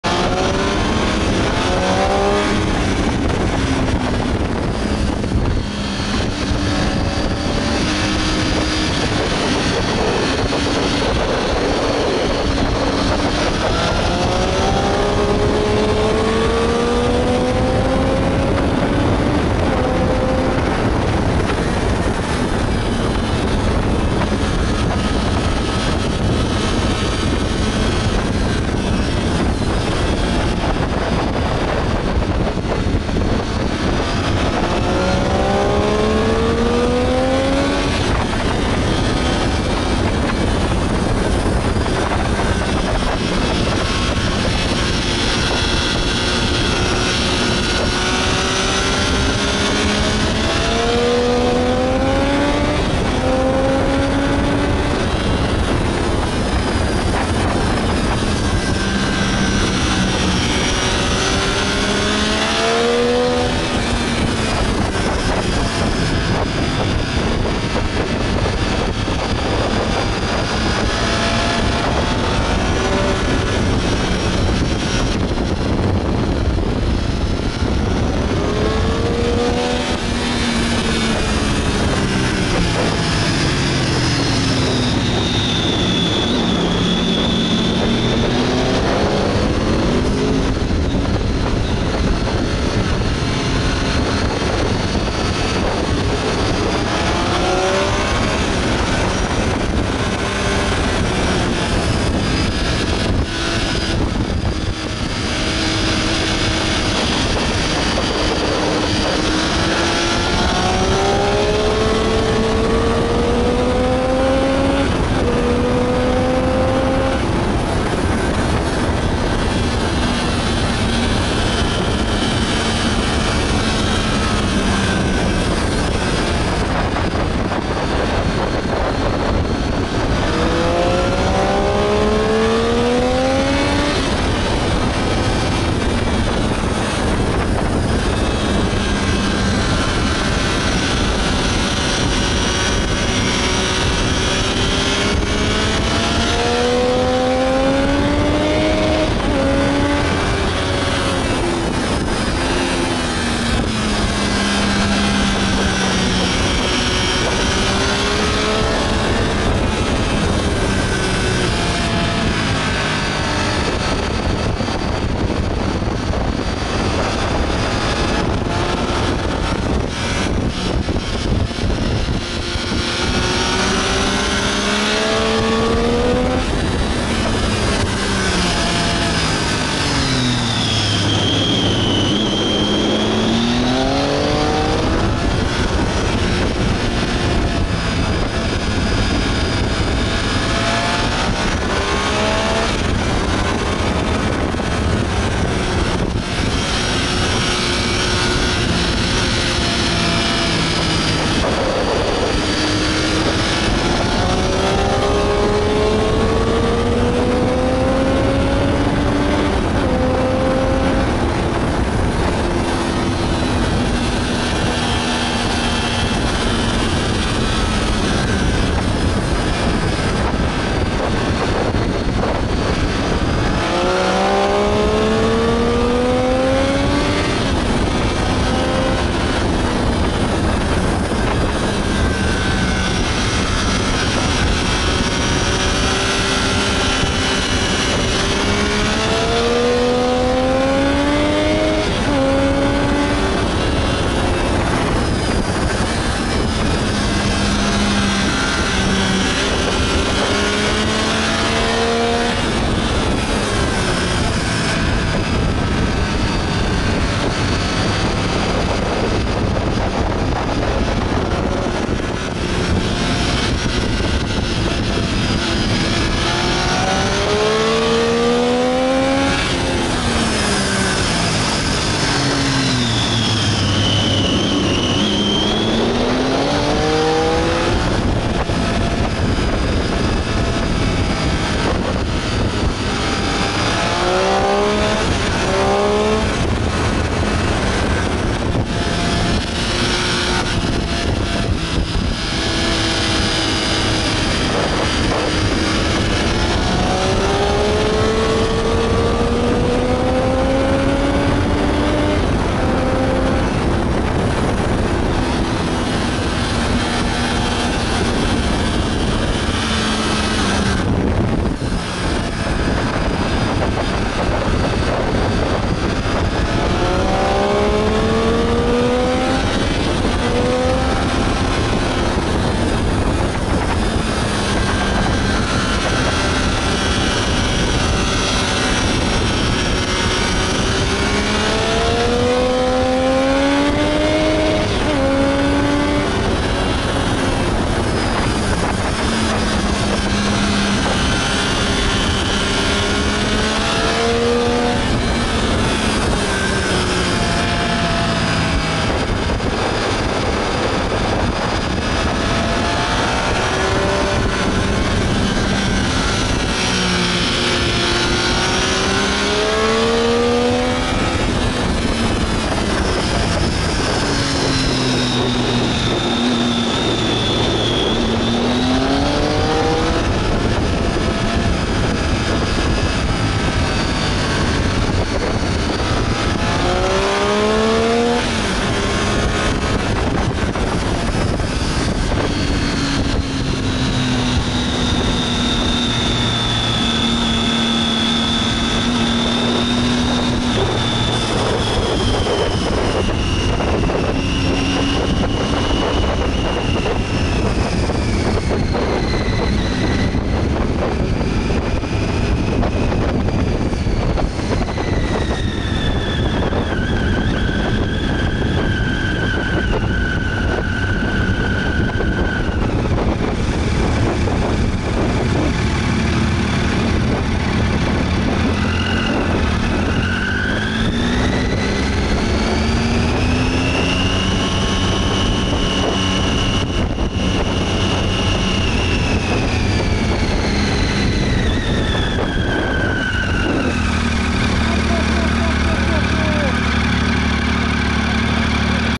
Session roulage groupé Alès juillet 2019
Super session avec un groupe de 5-6 motos. Ma vieille petite street à tenu le choc face aux RSV4, R1 et autre Panigales, elle est toujours vaillante :). Trop bon comme session!!!